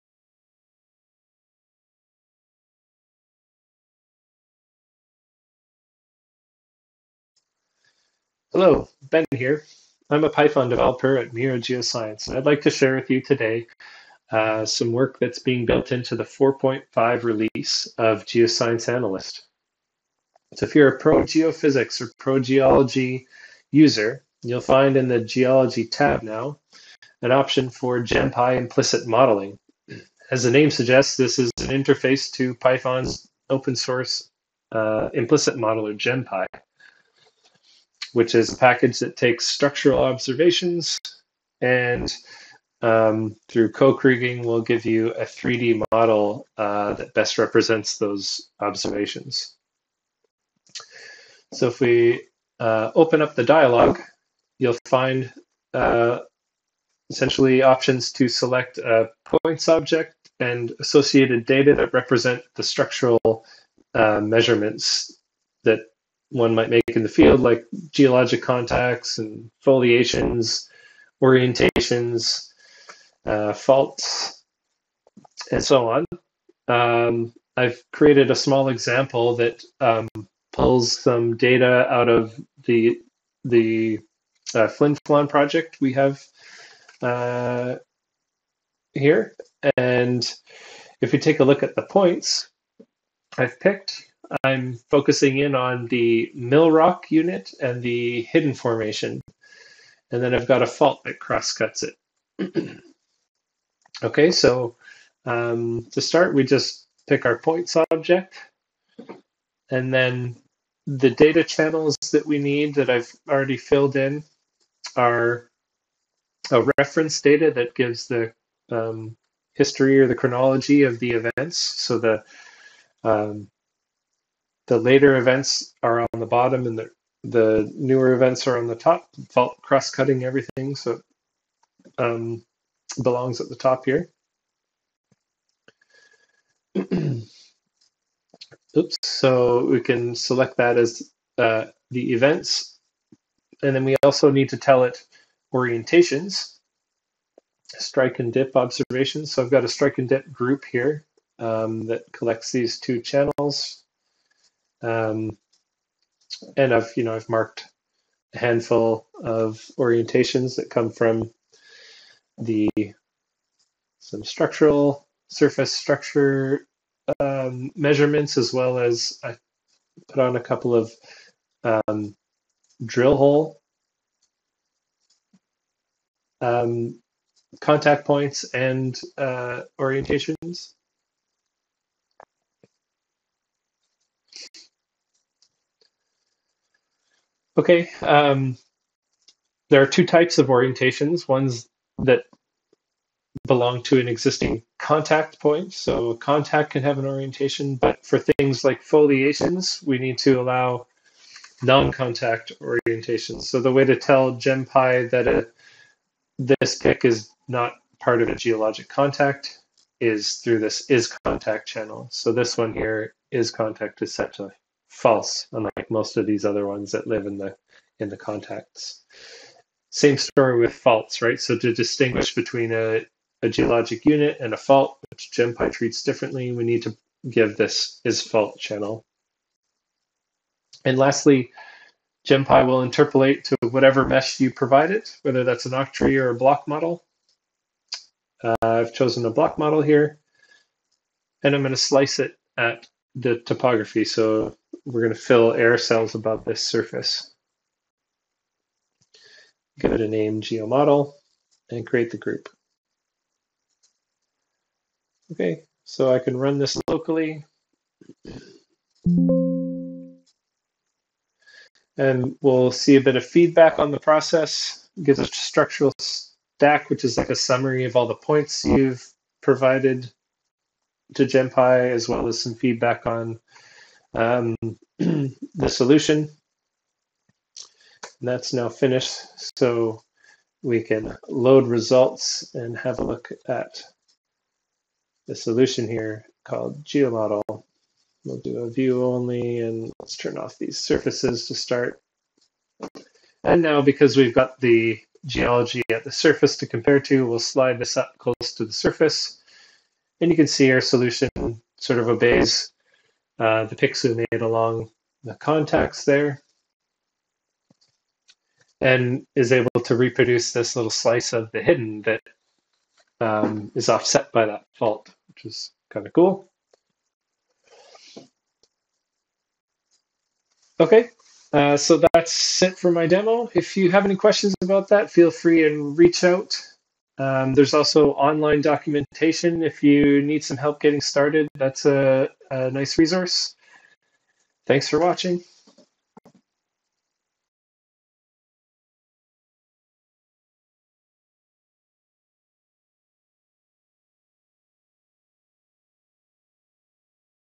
GemPy Implicit Modelling | Geoscience ANALYST Pro Geology and Pro Geophysics 4.5
Do you have 5 minutes? Watch this short video to see GemPy in action, now available in Geoscience ANALYST Pro Geology and Pro Geophysics!

GemPy is an open-source implicit modeller for rapidly creating and updating 3D geological models. Leverage the power of the Geoscience ANALYST platform as an interface to GemPy to define inputs, create and update 3D models, and undertake interpretation in the context of all your geoscience data.